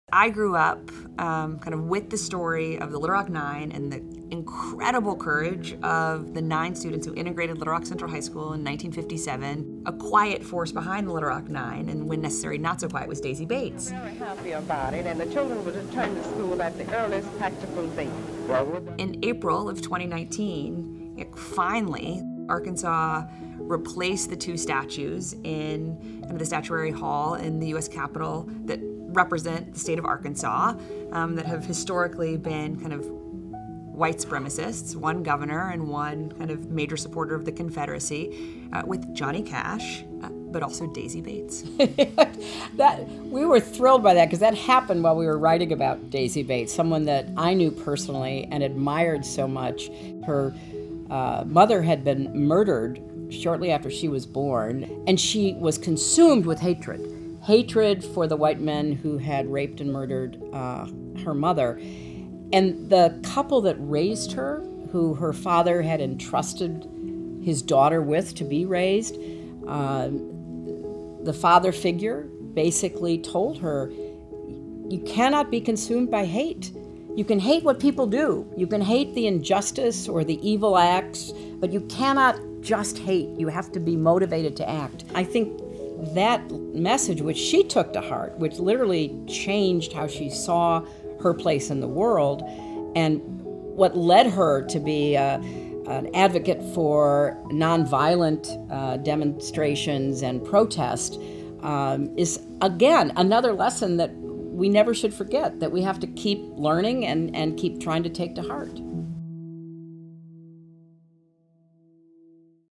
Daisy Bates: The Quiet Force Behind the Little Rock Nine
Hillary Rodham Clinton and her daughter, Chelsea, discuss gutsiness of civil rights activist Daisy Bates, whose profile is featured in the "The Book of Gutsy Women."

"You've probably seen the famous images from the 1950s of black students walking past crowds of angry white people to integrate schools after the Supreme Court ruled, in Brown v. Board of Education, that separate is not equal. ⁣⁣In Arkansas, a group of those pioneering students were known as the Little Rock Nine. They walked in to Central High School under troop escort 62 years ago this week. ⁣⁣I was lucky enough to know Daisy Bates, the Little Rock Nine's guide and advisor. A true gutsy woman, she taught me, and Chelsea, some lessons we'll never forget." –Secretary Clinton